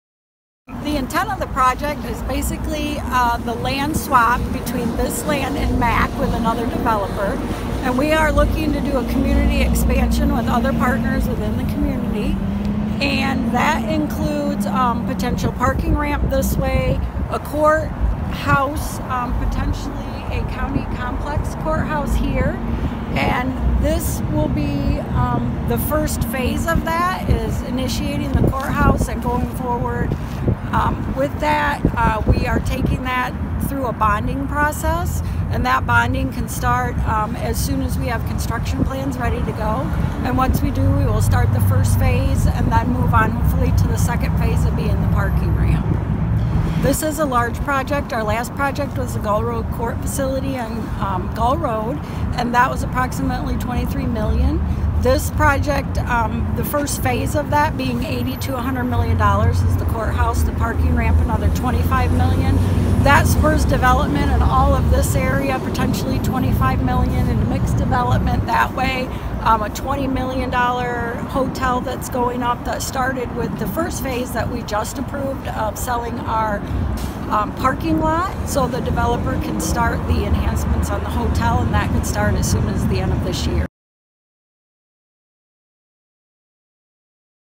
Kalamazoo County pursues downtown development
Kalamazoo County Administrator Tracie Moored speaks on Thursday, March 2, about potential downtown developments.